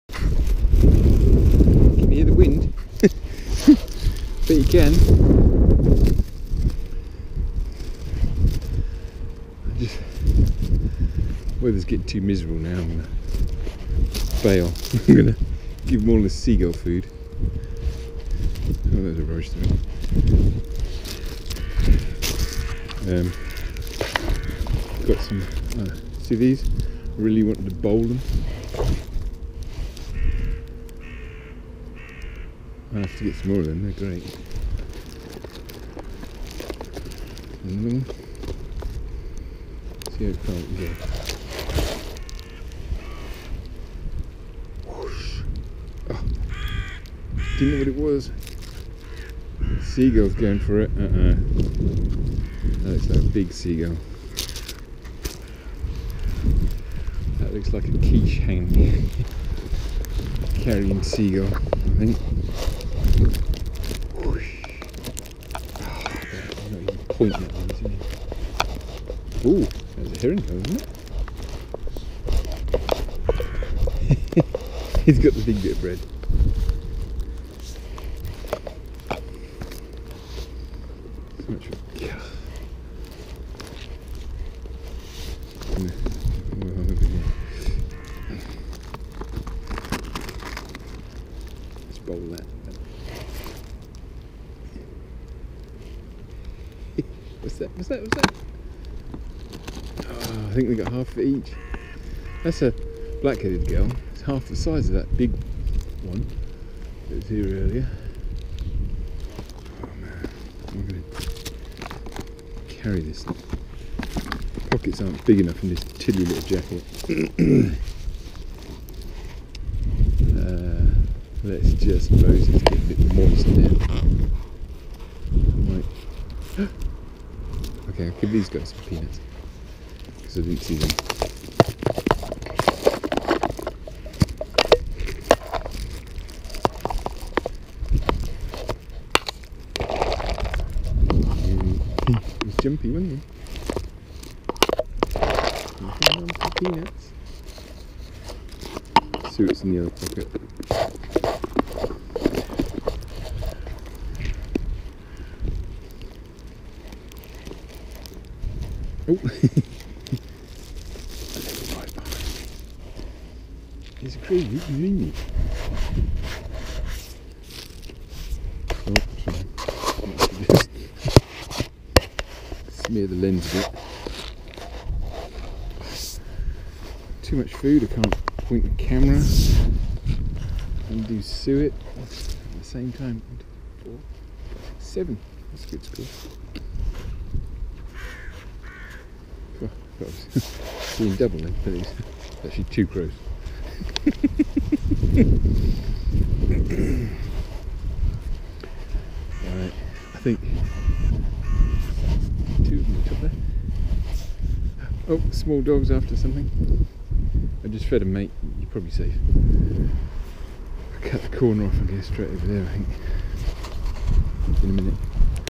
White Table Woods. feeding crows. 1779 peanuts in the rain
This year Ive taken to feeding the crows in the park at dinnertime. Some of the clips are amusing, but you might need to watch two or three to find a funny one. You can see the birds grow up during the year. Im learing about bird territories.  Birds can see in UV, what do crows look like to each other?